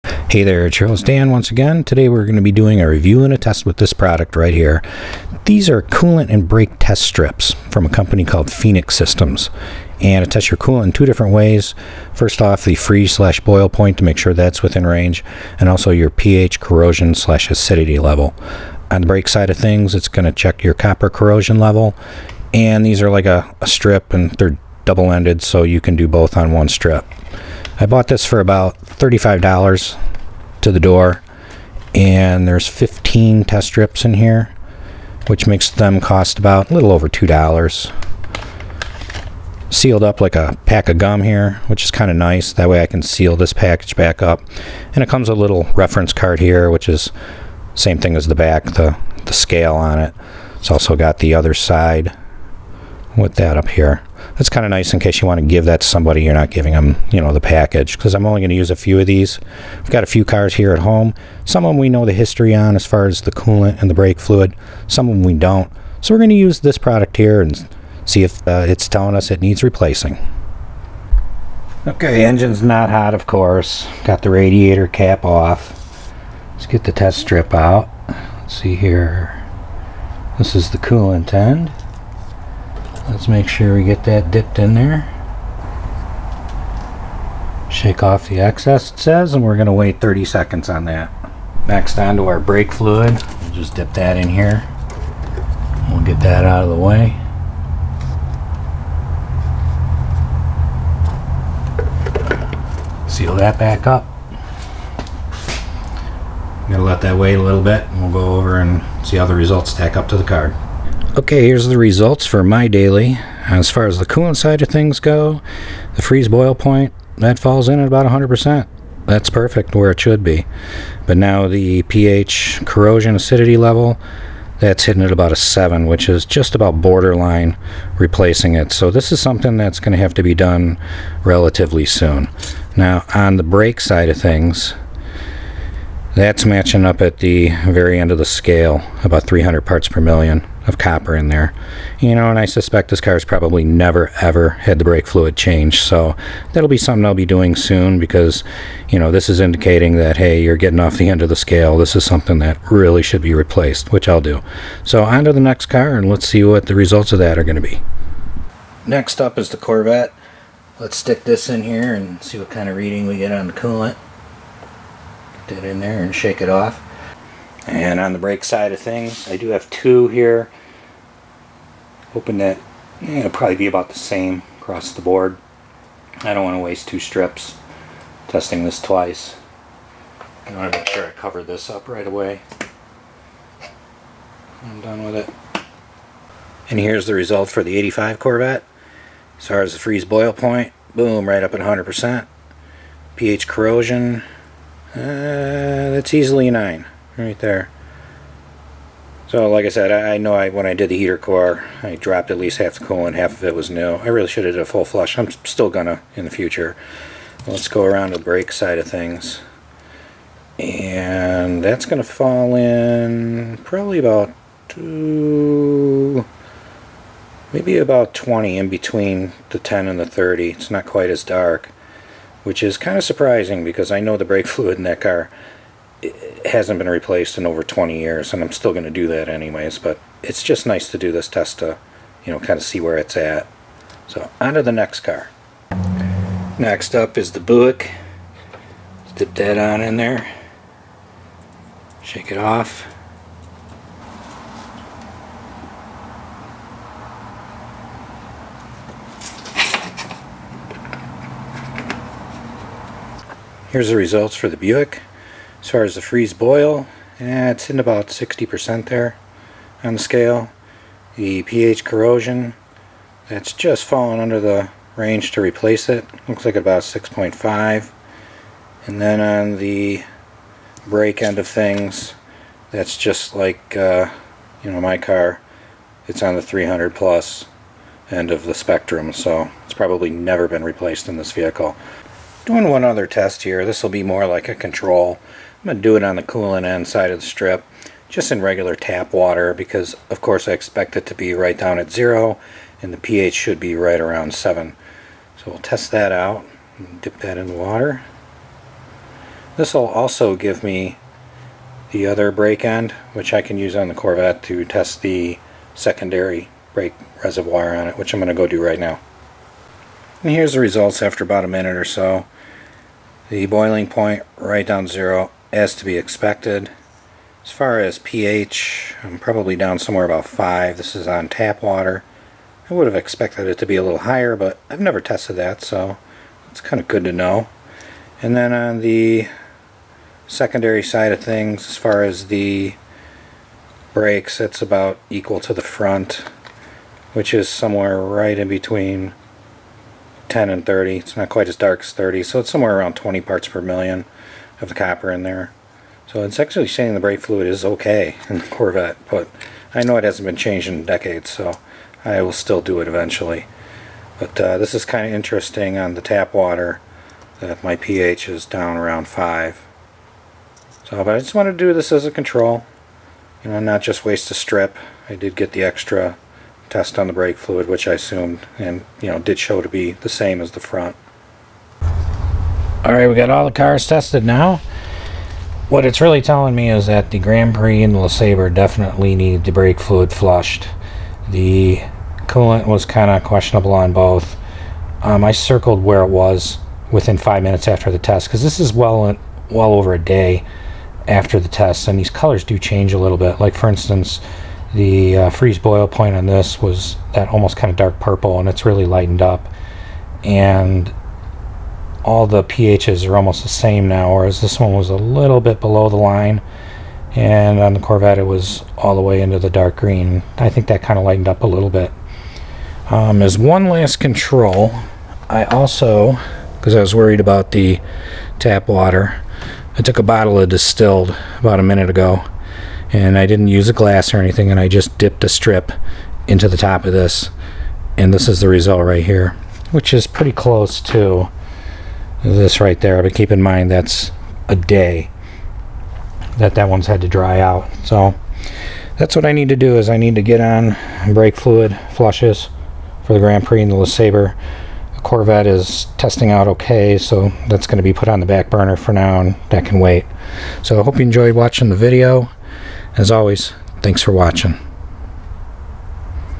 Coolant & Brake Fluid Testing Phoenix Systems Test Strip Review
A review of Phoenix Systems Coolant and Brake Fluid test strips.  Quickly and easily testf your fluids too check if they are in need of replacement.  Great product for testing a used car that comes with little or no maintenance history.